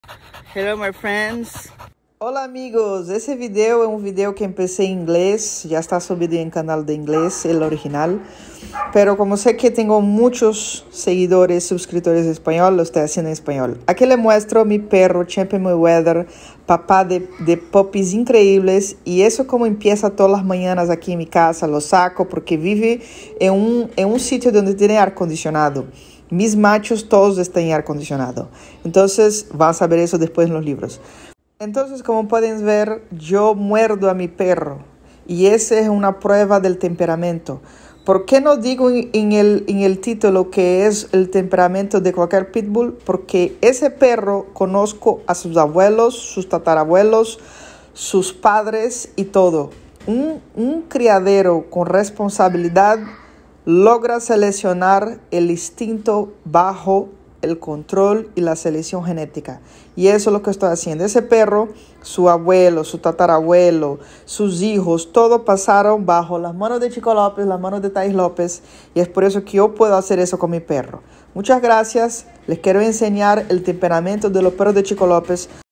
El TEMPERAMENTO del verdadero PITBULL de CHICO LOPEZ 🤩
Chico Lopez PITBULL TEMPERAMENT ♥️♥️♥️ I trust my dogs with my life - Dogs that loves humans 🤩🤩🤩